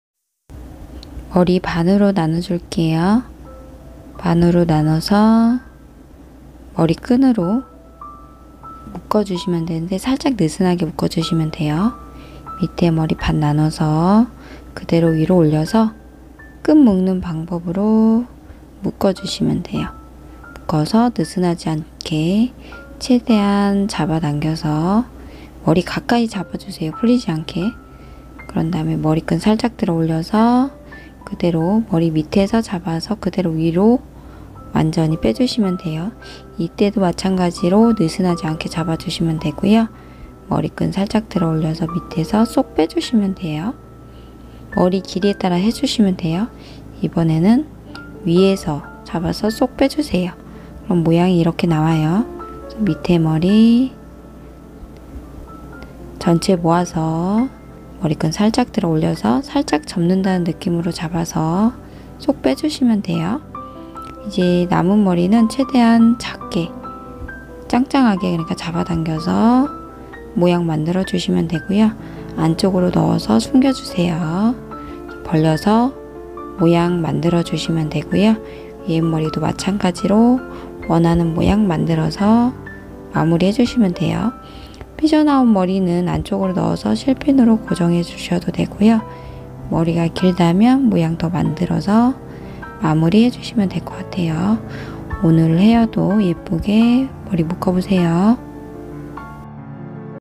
머리끈 하나로 묶어보는 올림머리 한복헤어 추천! /long hair easy updo
안녕하세요
머리하는 숑이 입니다
오늘은 머리끈 하나로 쉽고 예쁘게 머리묶기 해봤어요
쉽게 하려고 노력했어요

한복헤어 하셔도 너무 좋아요

묶으면서 올림머리 해봤어요

머리하기전
고데기 말고 시작해 주시면 좀더 자연스러운 올림머리 완성 됩니다

응용해서 도전 해보세요

남은 시간도 행복하세요